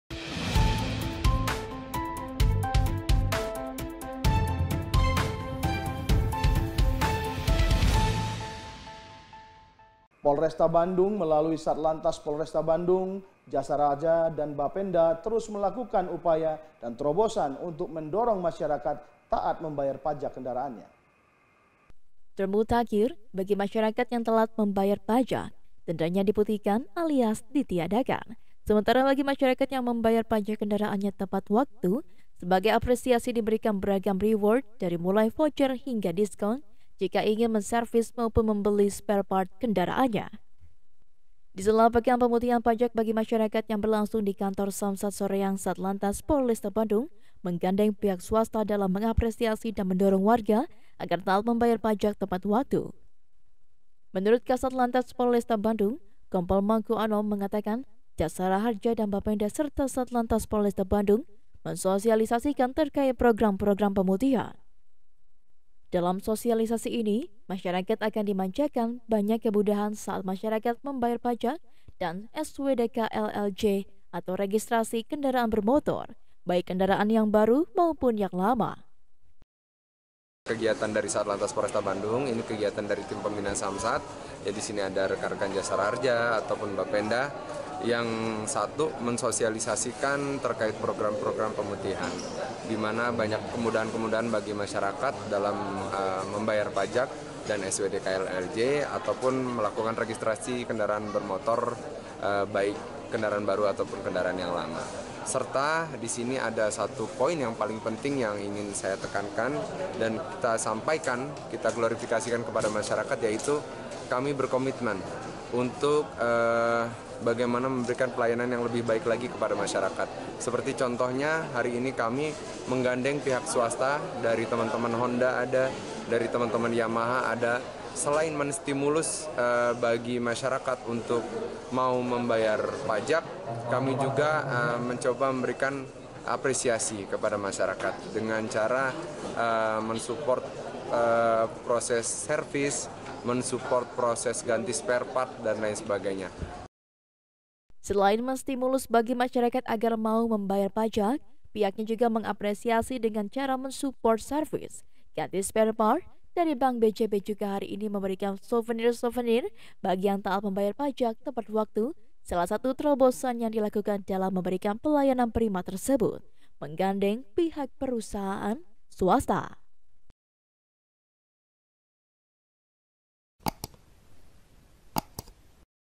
Diskon Servis Kendaraan dari Polresta, Begini Caranya ! | UN News | PTV
Polresta Bandung Melalui Satlantas Polresta Bandung, Jasa Raharja dan Bapenda Terus Melakukan Upaya dan Terobosan untuk Mendorong Masyarakat Taat Membayar Pajak Kendaraannya.

Simak Video Berikut untuk Berita Selengkapnya ya, Hanya di UN News PTV.